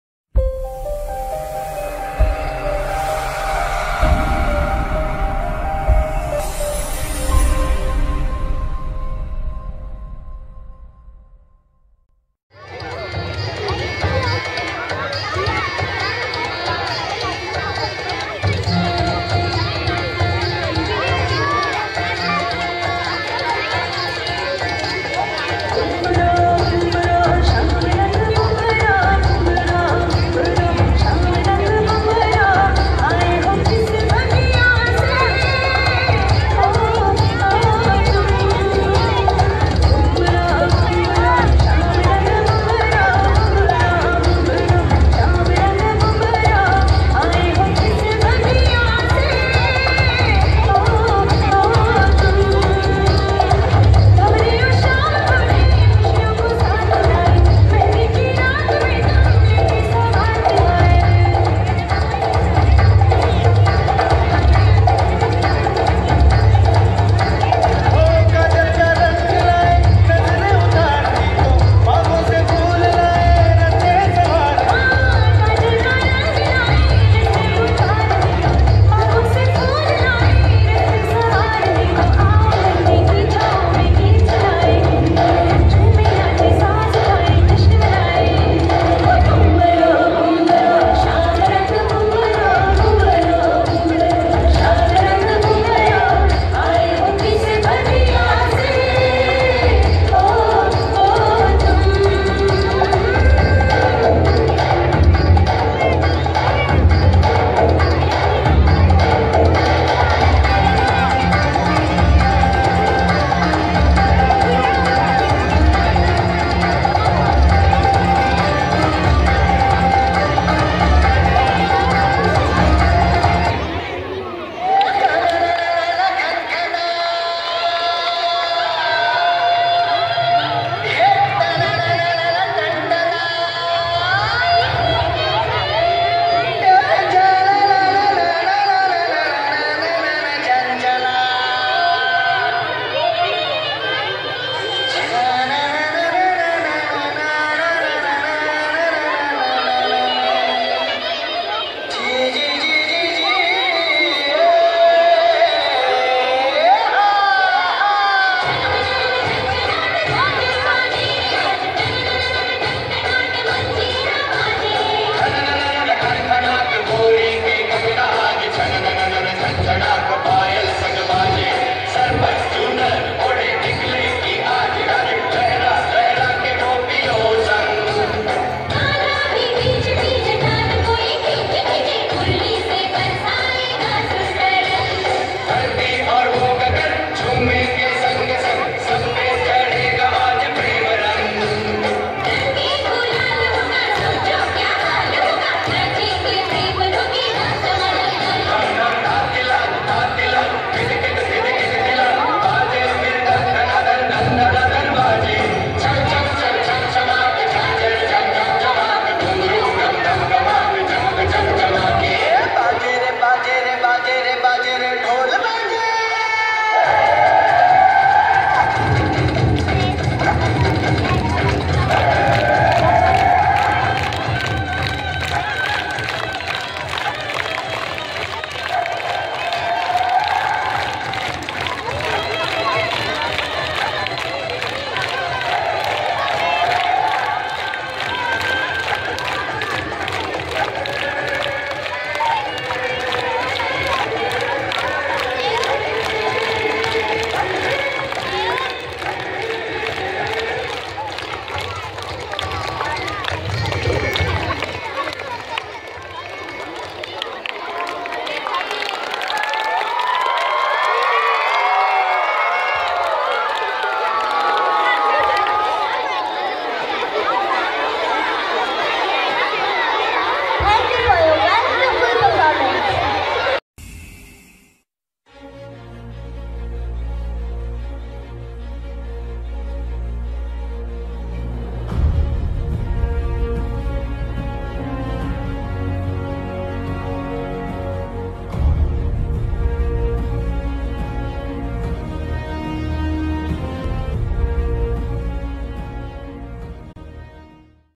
Folk Dance Competition_St.Stephen's School Dahod_HD Version
Beautiful Dance Performance @ ST.Stephen's School Dahod.